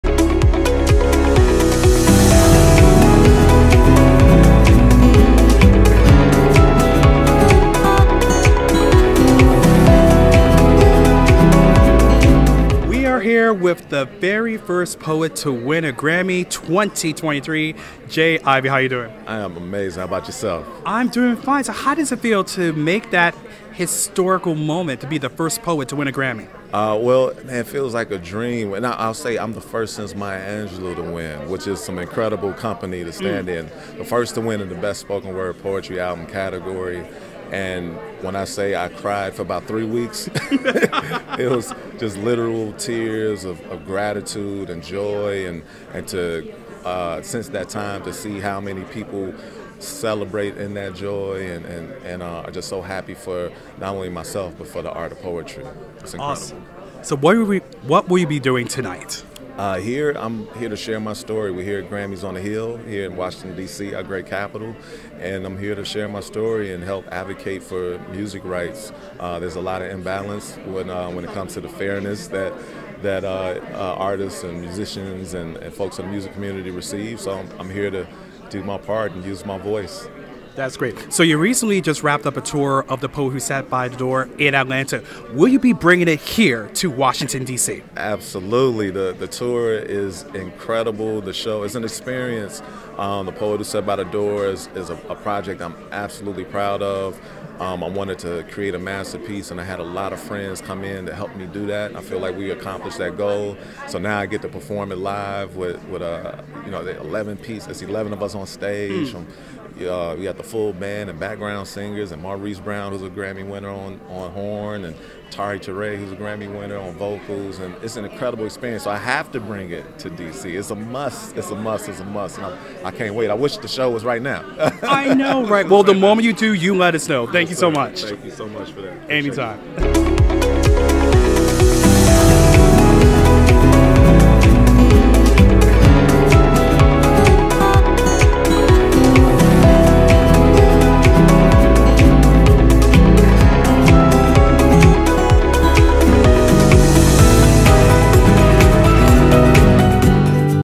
Grammys on The Hill 2023: J. Ivy
We like to interview people who make history and J. Ivy is no different! He's the first Grammy winner of the new "Best Spoken Word Poetry Album".